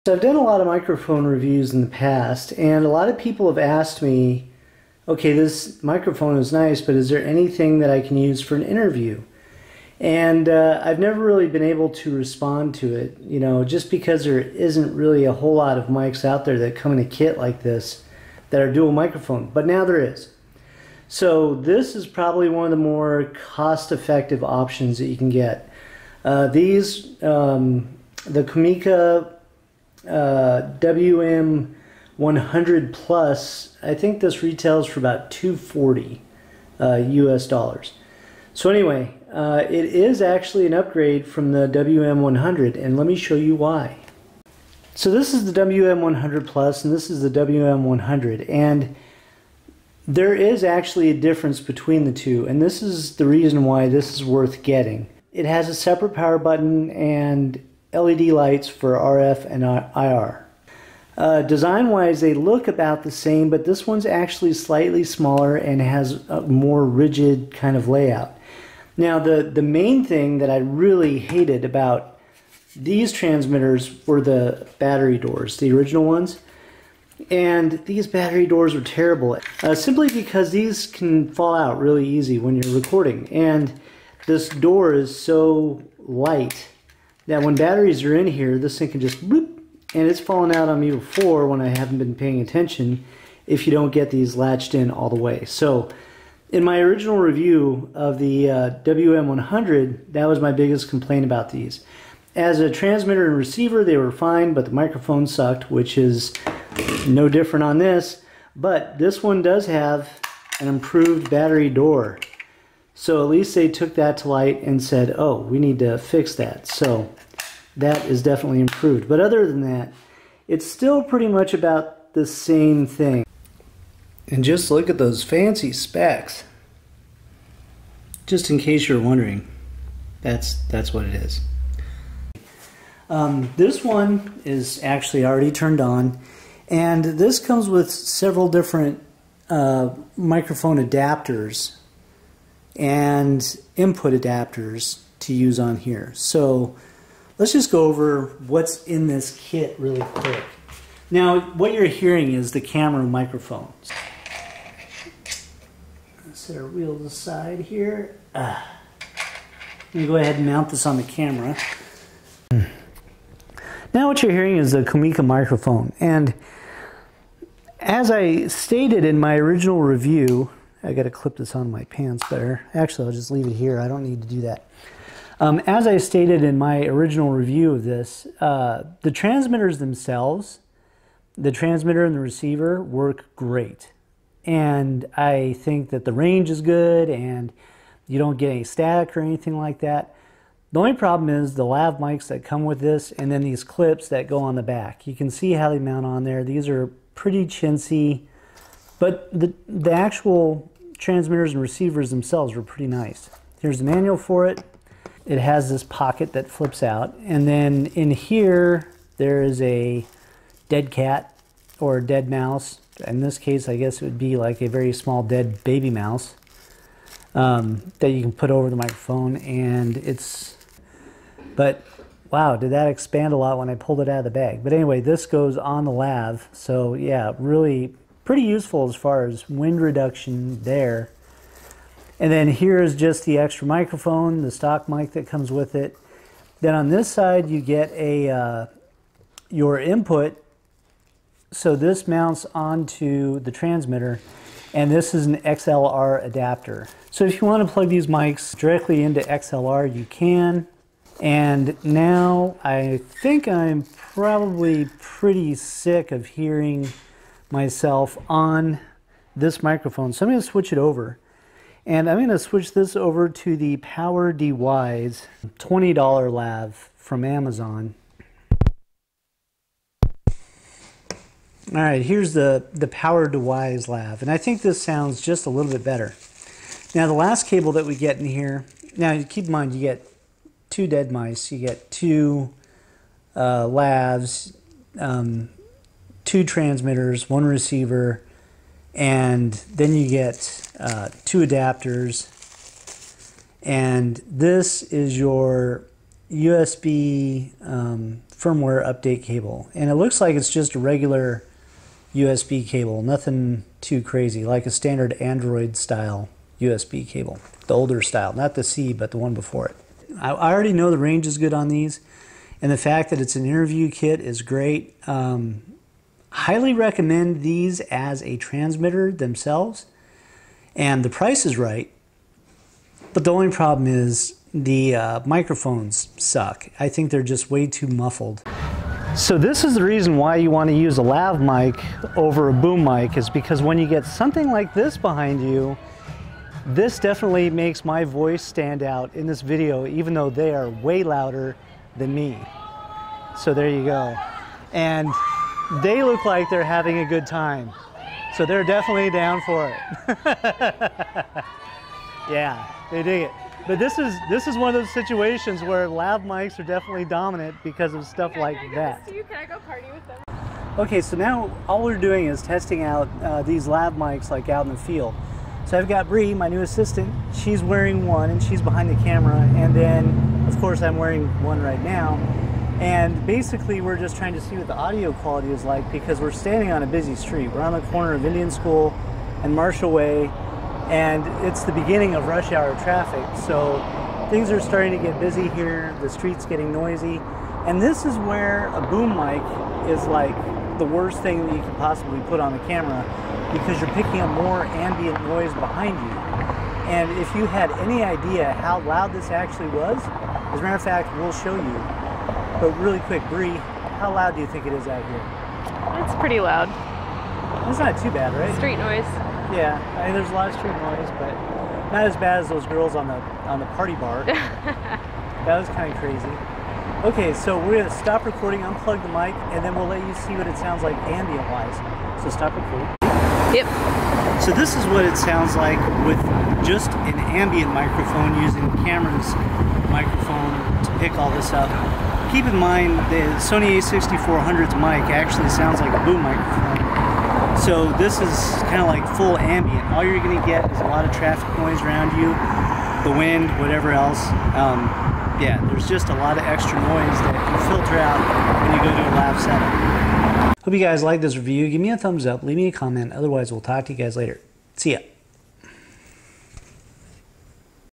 COMICA CVM-WM100 Plus Interview Wireless Lav Mic Kit Review
COMICA CVM-WM100 Plus Dual-Transmitter and One Receiver UHF 48-Channels Mono/Stereo Wireless Camere Lavalier Microphone Filmmaker Kit for Canon Nikon Panasonic Sony
Amazon Canada
Amazon UK

PowerDeWise mic:
Amazon USA
Amazon UK
Amazon Ca